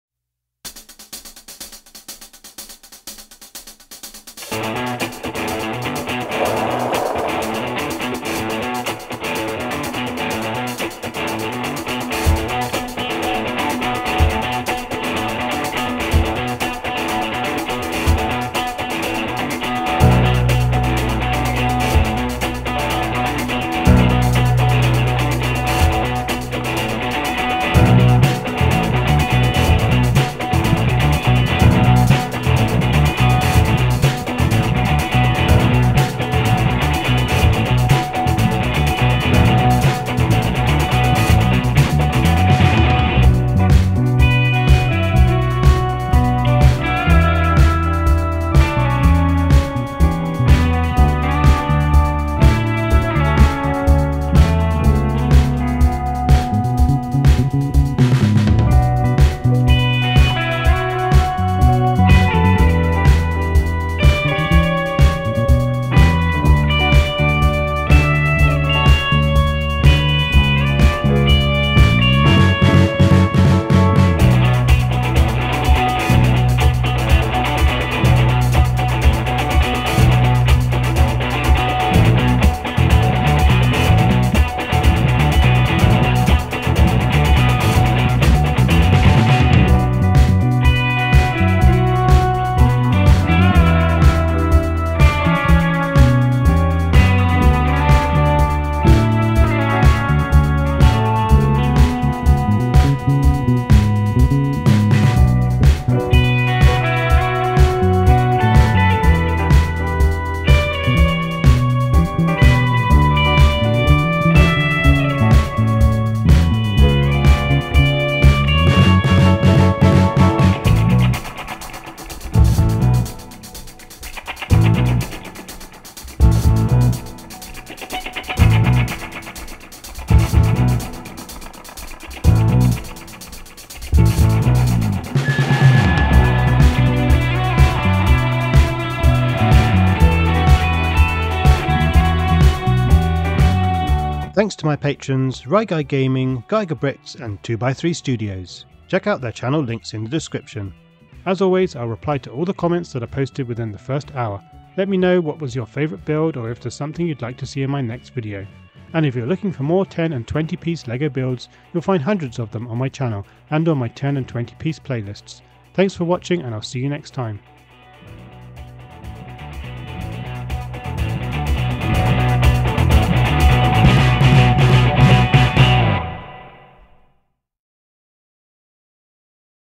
10 Tanks you can make with 10 Lego pieces or less
See how to make 10 tiny tanks. Please don't expect the highest level of accuracy and detail to each of the types of tank but I hope you enjoy the different ideas for how to make really tiny tanks using hardly any Lego pieces - great for your micro military brickfilm! In this video I make microscale and nanoscale MOCs using 10 pieces or less for each of a VK 45.01 (P) / Porsche Tiger Tank, a Panzerjäger Tiger (P) Ferdinand / Panzerabteilung 653, a Char B1, a Type 97 Chi-Ha 120mm Short Gun, a Panzer IV with 7.5cm KwK 37 L/24, a M1931 B-4 203-mm Howitzer, a Semovente 90-53 Scafo M41, a M41 Walker Bulldog, a 10.5cm K. gepanzerte Selbstfahrlafette Iva, and a Sturmtiger.

00:00 Intro
00:09 VK 45.01 (P) / Porsche Tiger Tank
00:26 Panzerjäger Tiger (P) Ferdinand / Panzerabteilung 653
00:41 Char B1
00:52 Type 97 Chi-Ha 120mm Short Gun
01:07 Panzer IV with 7.5cm KwK 37 L/24
01:20 M1931 B-4 203-mm Howitzer
01:33 Semovente 90-53 Scafo M41
01:48 M41 Walker Bulldog
02:01 10.5cm K. gepanzerte Selbstfahrlafette Iva
02:12 Sturmtiger
02:25 Thanks and outro

Special thanks to my Patrons RyGuyGaming, GeigerBricks, and 2x3 Studios for supporting me! Their channel links are here: